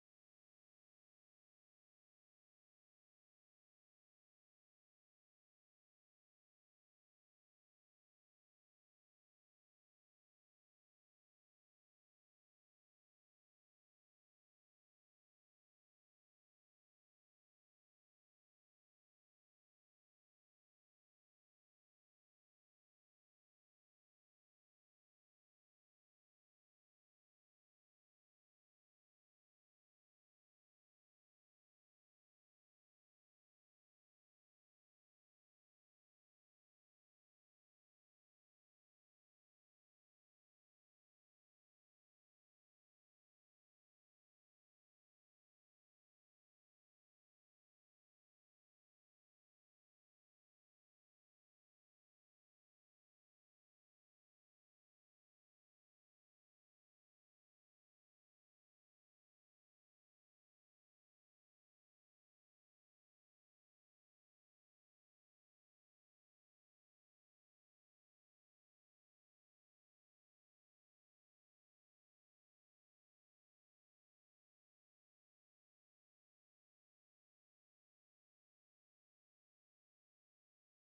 melted snow dripping down a drain pipe at 500fps
shot with the sony rx10iv